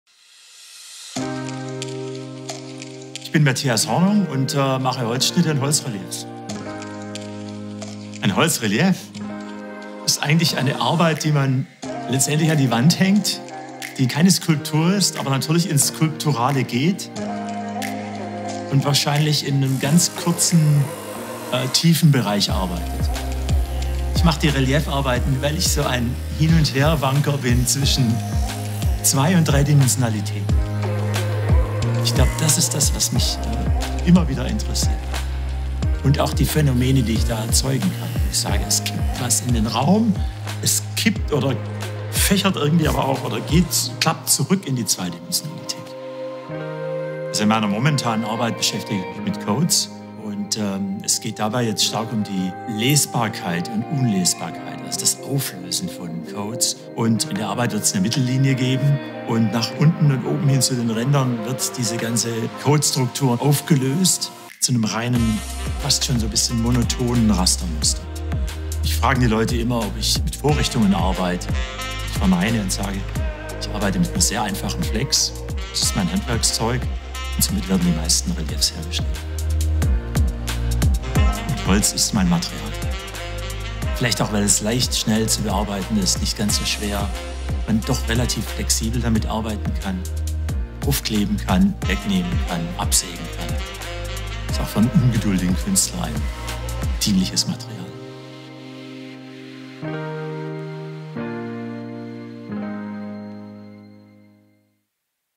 smart-collectors präsentiert Mathias Hornung
Mathias Hornung: kreativer Geist und Maler aus Berlin, Deutschland.

Dein Zugang zur Kunst. Deine ästhetische, alternative Anlageform, mit der Du Deinen eigenen Stil definierst.

Kameramann: Matthias Knebl
Ton: Ben Kaufmann